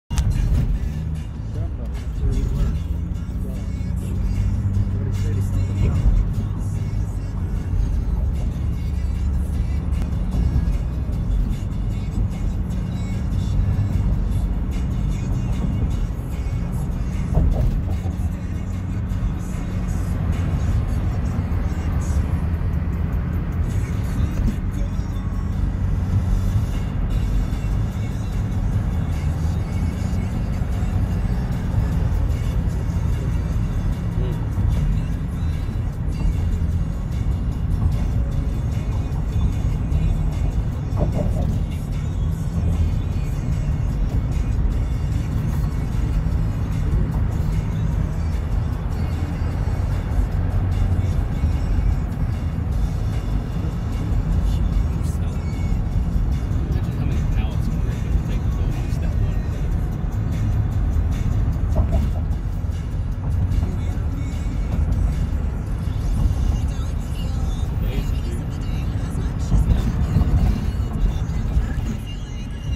Brooklyn Bridge 11-5-19
Crossing from Brooklyn to Manhattan.